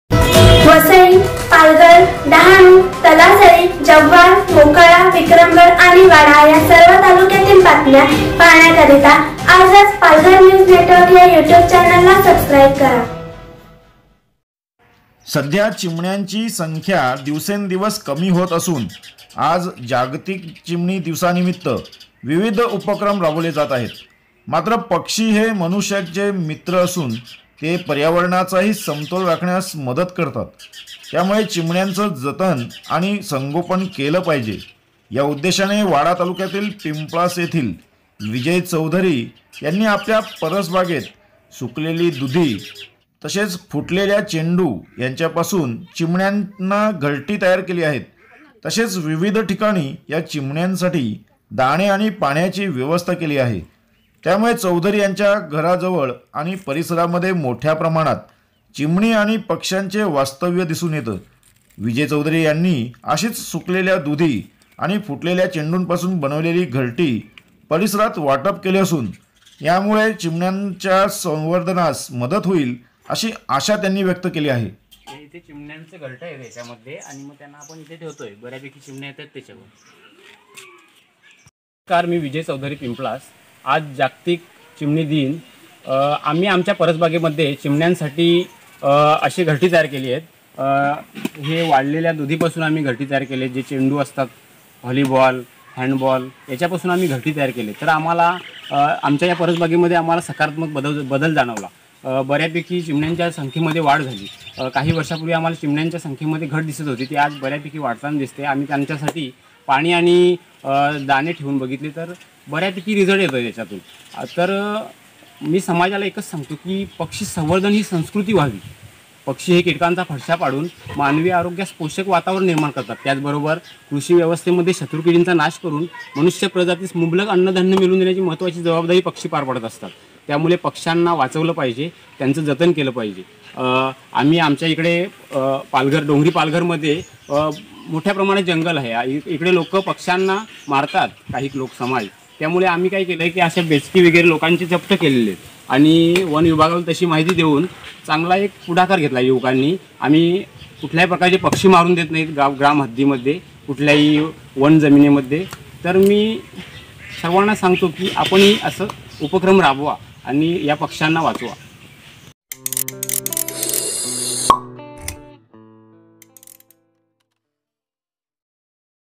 वाडा॥जागतिक चिमणी दिवस॥संवर्धक विजय चौधरी पक्षीप्रेमींचे अजब प्रेम
सध्या चिमण्यांची संख्या दिवसेंदिवस कमी होत असून आज जागतिक चिमणी दिवसानिमित्त विविध उपक्रम राबविले जात आहेत .  मात्र पक्षी हे मनुष्यांचे मित्र असून ते पर्यावरणाचाही समतोल राखण्यास मदत करतात . त्यामुळे चिमण्यांच जतन आणि संगोपन केल पाहिजे या उद्देशाने वाडा तालुक्यातील पिम्प्लास येथील विजय चौधरी यांनी आपल्या परसबागेत सुकलेले दुधी , फुटलेल्या चेंडू यांच्यापासून चिमण्यांना घरटी तयार केली आहेत . तसेच विविध ठिकाणी या चिमण्यांसाठी दाणे आणि पाण्याची व्यवस्था केली आहे . त्यामुळे चौधरी यांच्या घर आणि परिसरामध्ये मोठ्या प्रमाणात चिमणी आणि पक्ष्यांचे वास्तव्य दिसून येतं,विजय चौधरी यांनी अशीच सुकलेल्या दुधी आणि फुटलेल्या चेंडूंपासून बनवलेली घरटी परिसरात वाटप केले असून यामुळे चिमण्यांच्या संवर्धनास मदत होईल अशी आशा त्यांनी व्यक्त केले

बाईट-विजय चौधरी,,पक्षी प्रेमी,संवर्धक